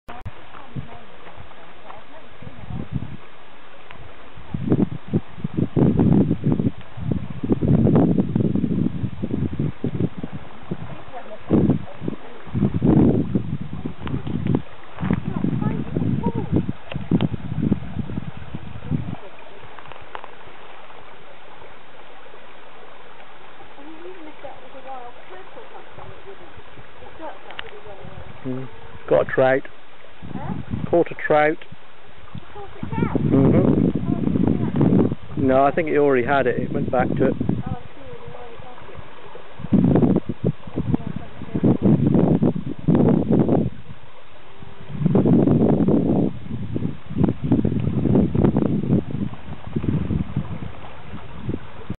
Mink Near Applecross
A short wobbly clip of an American mink (Neovison vison) fishing for brown trout in daytime near the mouth of a small burn south of Applecross in the Northwest Highlands.
Sorry for crappy quality, the sun on the screen on the back of the little camera meant I couldn't really tell where I was filming.